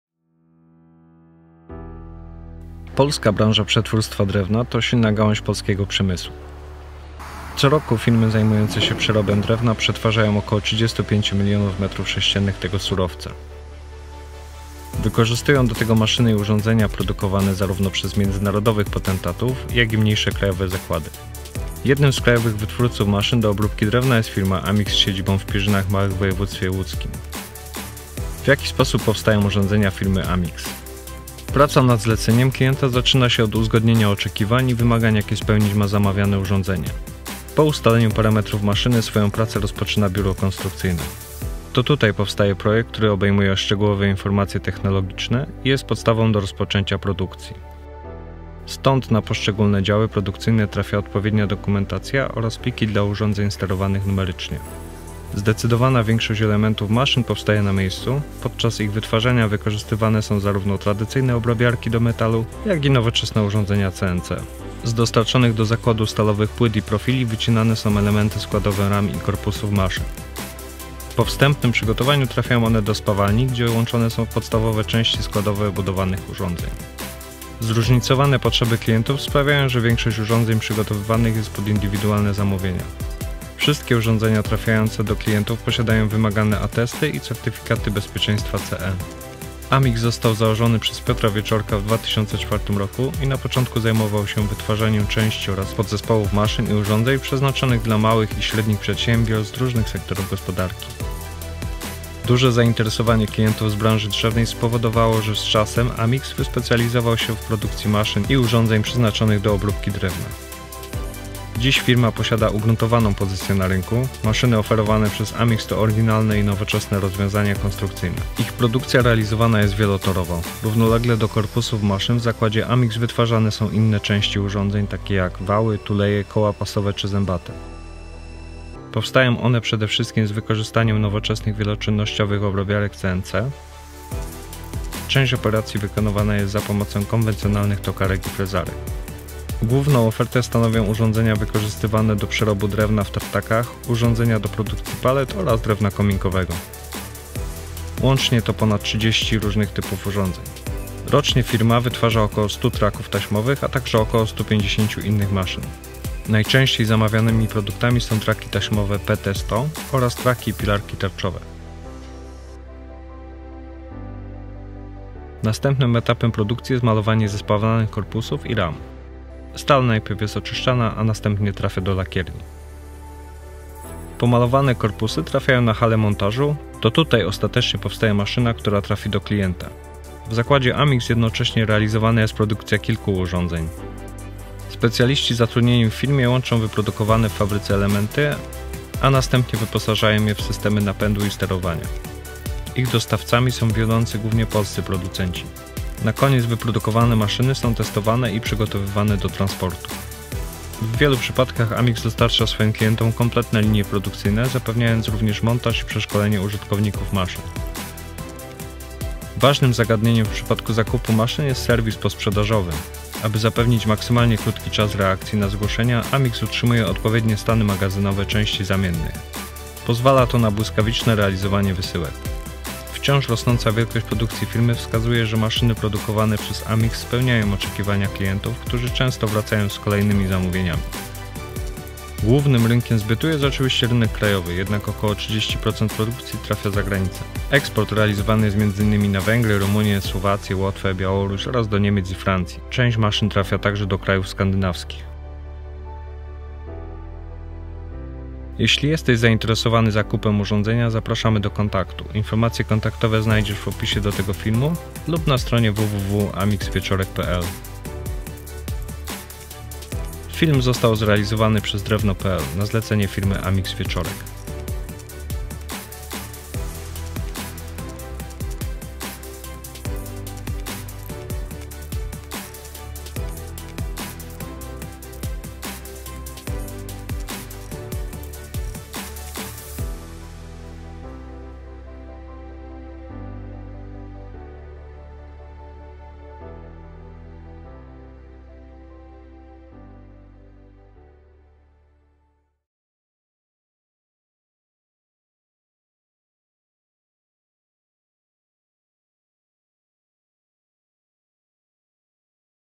Jak powstają maszyny do przerobu drewna produkowane przez firmę Amix
Polska branża przetwórstwa drewna to silna gałąź polskiego przemysłu. Co roku firmy zajmujące się przerobem drewna przetwarzają ok. 35 mln m3 tego surowca. Wykorzystują do tego maszyny i urządzenia produkowane zarówno przez międzynarodowych potentatów, jak i mniejsze krajowe zakłady. Jednym z krajowych wytwórców maszyn do obróbki drewna jest firma AMIX z siedzibą w Pierzynach Małych w woj. łódzkim.

W jaki sposób powstają urządzenia firmy AMIX?

Praca nad zleceniem klienta zaczyna się od uzgodnienia oczekiwań i wymagań jakie spełniać ma zamawiane urządzenie. Po ustaleniu parametrów maszyny swoją pracę rozpoczyna biuro konstrukcyjne.

To tutaj powstaje projekt, który obejmuje szczegółowe informacje technologiczne i jest podstawą do rozpoczęcia produkcji. Stąd na poszczególne działy produkcyjne trafia odpowiednia dokumentacja oraz pliki dla urządzeń sterowanych numerycznie.

Zdecydowana większość elementów maszyn powstaje na miejscu. Podczas ich wytwarzania wykorzystywane są zarówno tradycyjne obrabiarki do metalu, jak i nowoczesne urządzenia CNC.

Z dostarczonych do zakładu stalowych płyt i profili wycinane są elementy składowe ram i korpusów maszyn.

Po wstępnym przygotowaniu trafiają one do spawalni, gdzie łączone są w podstawowe części składowe budowanych urządzeń.

Zróżnicowane potrzeby klientów sprawiają, że większość urządzeń przygotowywanych jest pod indywidualne zamówienia. Wszystkie urządzenia trafiające do klientów posiadają wymagane atesty i certyfikaty bezpieczeństwa CE.

Amix został założony przez Piotra Wieczorka w 2004 roku i na początku zajmował się wytwarzaniem części oraz podzespołów maszyn i urządzeń przeznaczonych dla małych i średnich przedsiębiorstw z różnych sektorów gospodarki. Duże zainteresowanie klientów z branży drzewnej spowodowało, że z czasem AMIX wyspecjalizował się w produkcji maszyn i urządzeń przeznaczonych do obróbki drewna.

Dziś firma posiada ugruntowaną pozycję na rynku. Maszyny oferowane przez AMIX to oryginalne i nowoczesne rozwiązania konstrukcyjne. Główną ofertę stanowią urządzenia wykorzystywane do przerobu drewna w tartakach, urządzenia do produkcji palet oraz drewna kominkowego. Łącznie to ponad 30 różnych typów urządzeń.

Ich produkcja realizowana jest wielotorowo. Równolegle do korpusów maszyn w zakładzie AMIX wytwarzane są inne części urządzeń, takie jak wały, tuleje, koła pasowe czy zębate. Powstają one przede wszystkim z wykorzystaniem nowoczesnych, wieloczynnościowych obrabiarek CNC. Część operacji wykonywana jest za pomocą konwencjonalnych tokarek i frezarek.

Rocznie firma wytwarza ok. 100 traków taśmowych, a także około 150ciu innych maszyn. Najczęściej zamawianymi produktami są traki taśmowe PT100 oraz traki i pilarki tarczowe.

Głównym rynkiem zbytu jest oczywiście rynek krajowy, jednak około 30% produkcji trafia zagranicę. Eksport realizowany jest m.in. na Węgry, Rumunię, Słowację, Łotwę, Białoruś oraz do Niemiec i Francji, część maszyn trafia także do krajów skandynawskich.

Następnym etapem produkcji jest malowanie zespawanych korpusów i ram. Stal najpierw jest oczyszczana, a następnie trafia do lakierni.

Pomalowane korpusy trafiają na halę montażu. To tutaj ostatecznie powstaje maszyna, która trafi do klienta. W zakładzie Amix jednocześnie realizowana jest produkcja kilku urządzeń.

Specjaliści zatrudnieni w firmie łączą wyprodukowane w fabryce elementy, a następnie wyposażają je w systemu napędu i sterowania. Ich dostawcami są wiodący, głównie polscy producenci.

Na koniec wyprodukowane maszyny są testowane i przygotowywane do transportu.

W wielu przypadkach AMIX dostarcza swoim klientom kompletne linie produkcyjne, zapewniając również montaż i przeszkolenie użytkowników maszyn.

Ważnym zagadnieniem w przypadku zakupu maszyn jest serwis posprzedażowy. Aby zapewnić maksymalnie, krótki czas reakcji na zgłoszenia, AMIX utrzymuje odpowiednie stany magazynowe części zamiennych. Pozwala to na błyskawiczne realizowanie wysyłek.

Wciąż rosnąca wielkość produkcji firmy wskazują, że maszyny produkowane przez AMIX spełniają oczekiwania klientów, którzy często wracają z kolejnymi zamówieniami.

Jeśli jesteś zainteresowany zakupem urządzenia, zapraszamy do kontaktu. Informacje kontaktowe znajdziesz w opisie do tego filmu lub na stronie

Film został zrealizowany przez drewno.pl na zlecenie firmy Amix-Wieczorek

P.H.U. AMIX®

98-358 Kiełczygłów
Pierzyny Małe 5